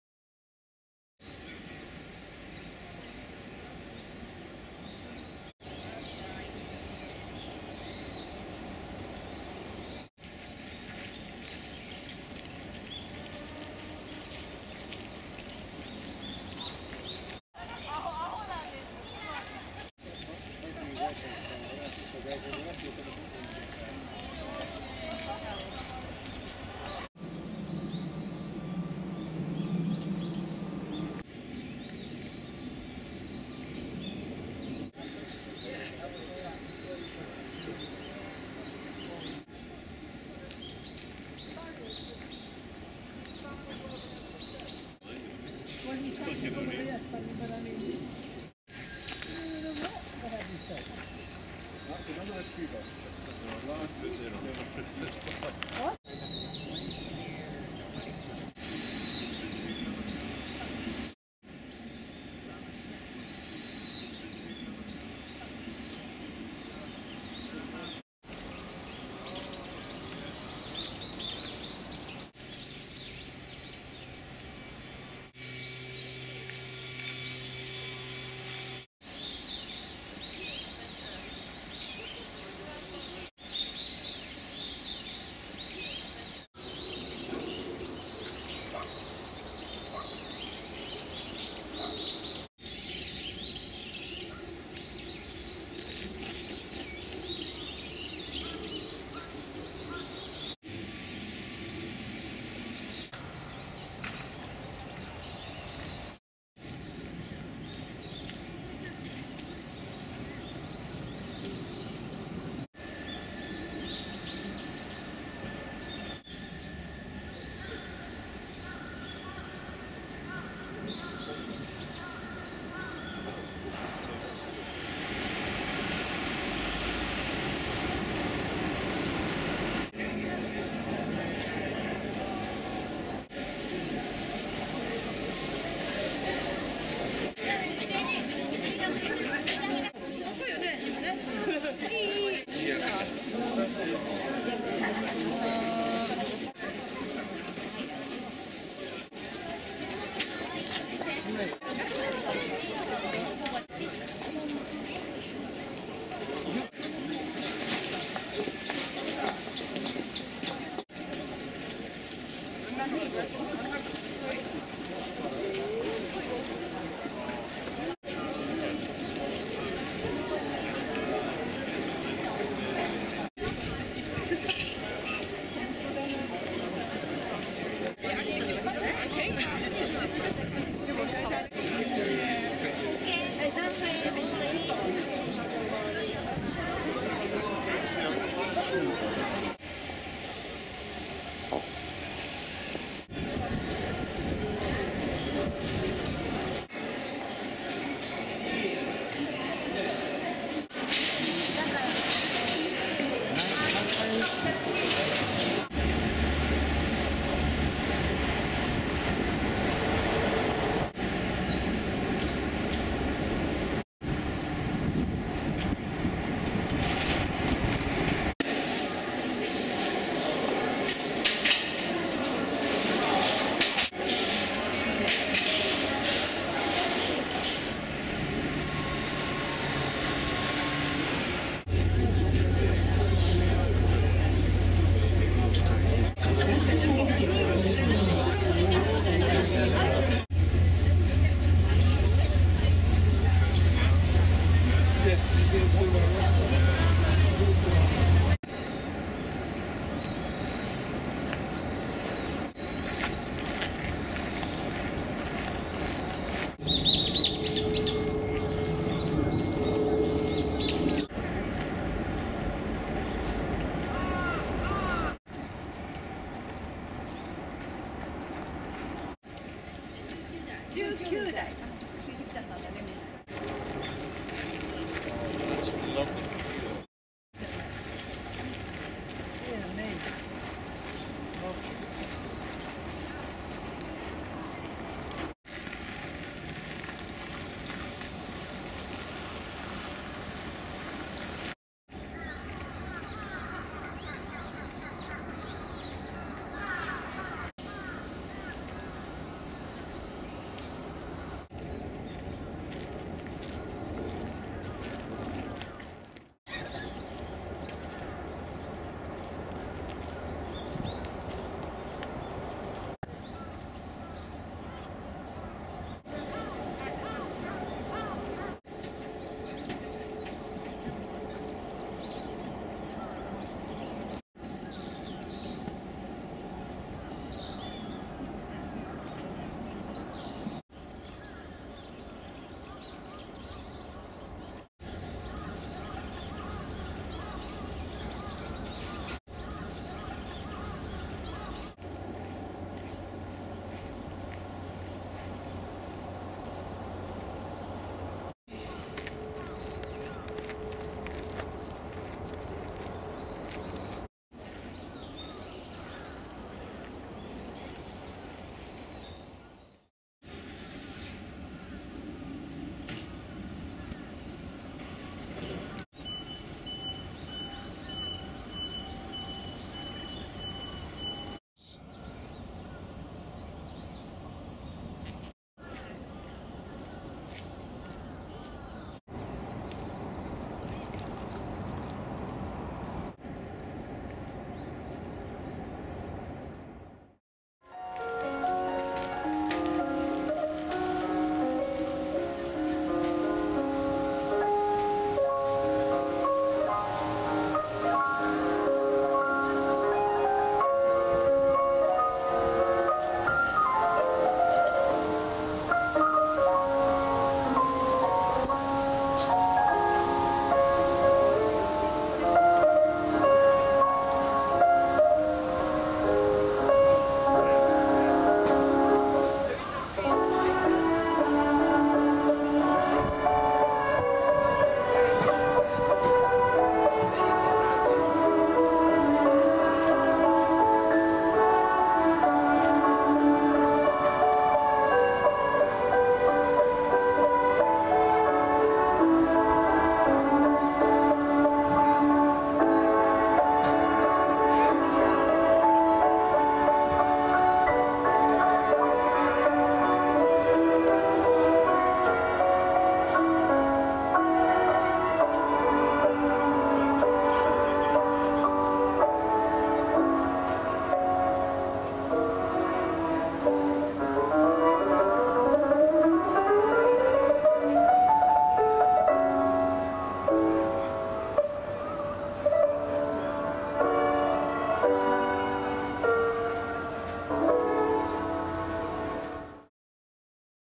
JAPAN - GARDENS & CULTURE (Part 8/12) - SENSO-JI TEMPLE and HAMARIKYU GARDENS
(All 12 parts Now based in Tokyo, we visit a superb stroll garden, then Tokyo's most sacred temple and finally a public park which was once the family garden of the Tokugawa Shogun. (14 November 2012) Part 9